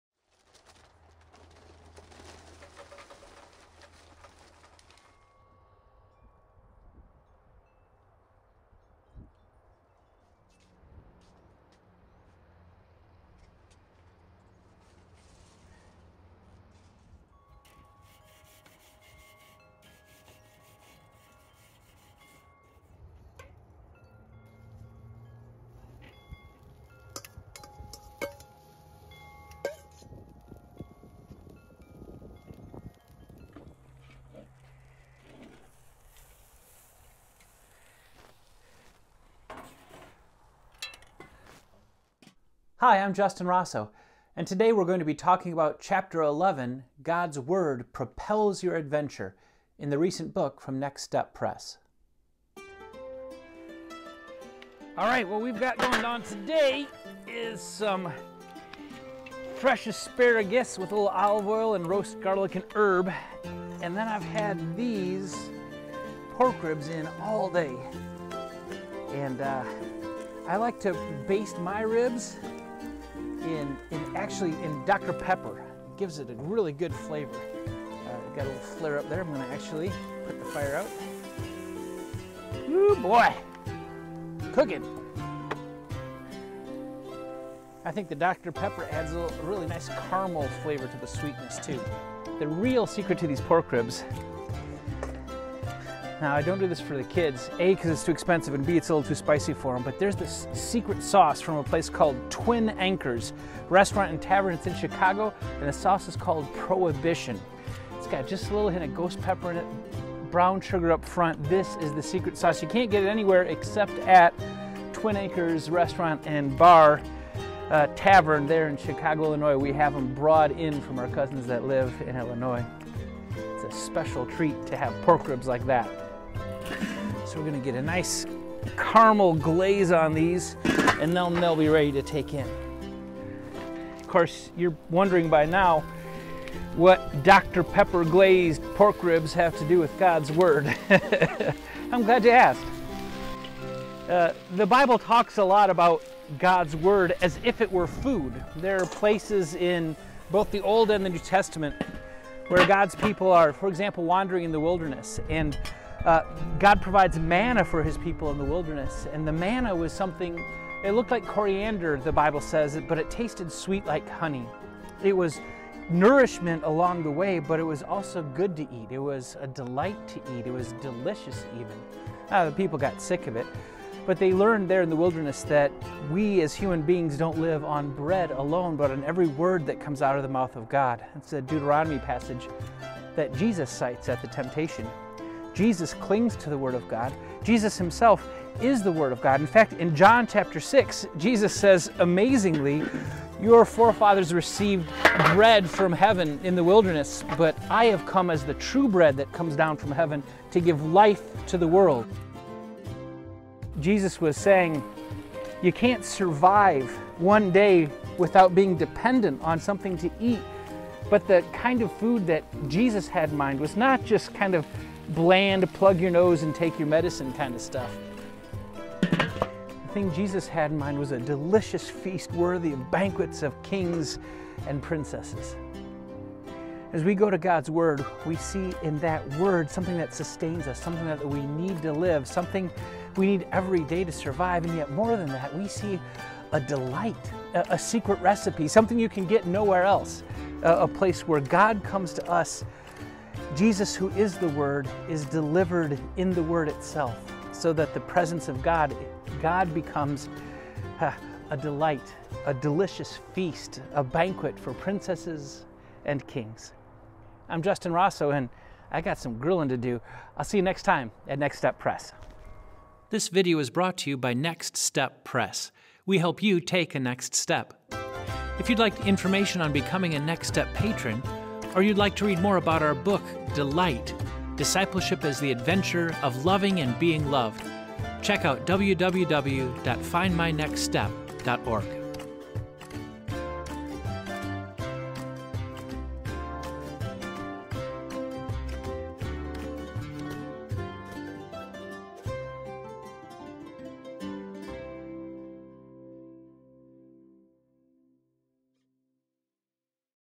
Delight! Video Intro to Chapter 11, God's Word Propels Your Adventure
We need God's Word to sustain this journey of faith. But ingesting God's Word isn't like taking your medicine; God' presence in the Word is a Delicious Delight!

Justin Rossow introduces Delight! Chapter 11: God's Word Propels Your Adventure, and gives away a few grilling secrets in the process. Even better than the manna that kept Israel alive in the wilderness, Jesus is the true bread from heaven who gives life to the world.

Find out more about the award-winning book Delight! Discipleship as the Adventure of Loving and Being Loved here:

This video is (c) Justin Rossow and Next Step Press, 2021.